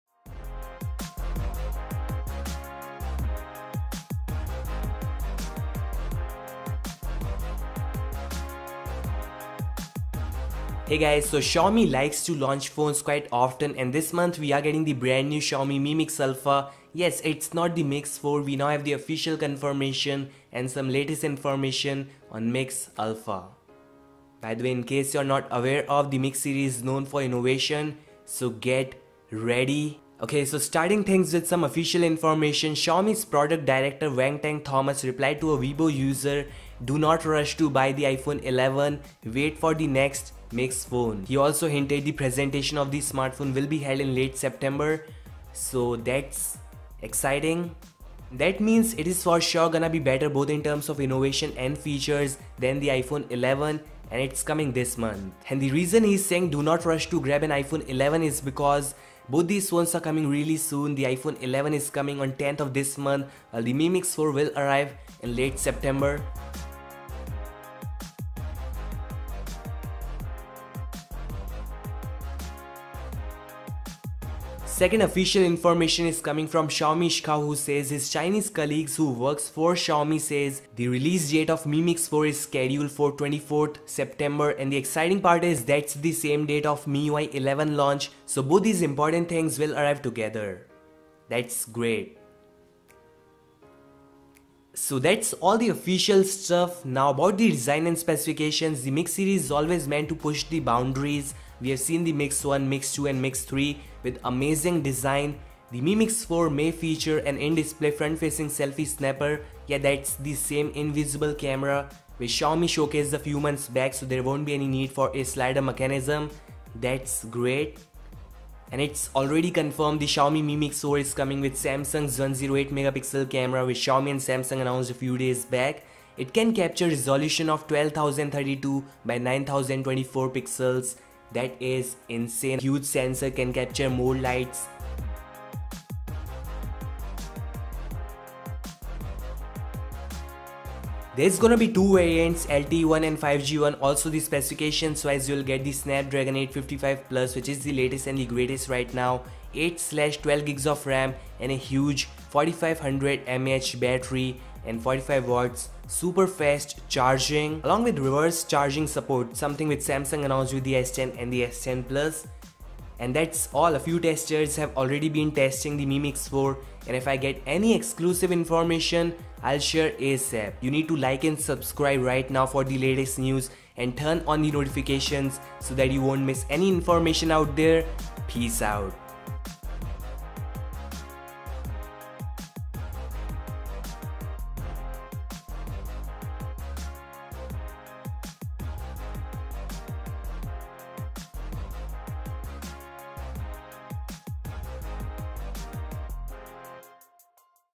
Xiaomi Mi MIX 4 (MIX α) - GET READY!!!
Xiaomi Mi Mix Alpha α AKA Mi MIX 4 is Coming on September 24th With all the latest and greatest features, World's First 108MP Camera.

MIX 4 Features, Specifications, Design, and Release Date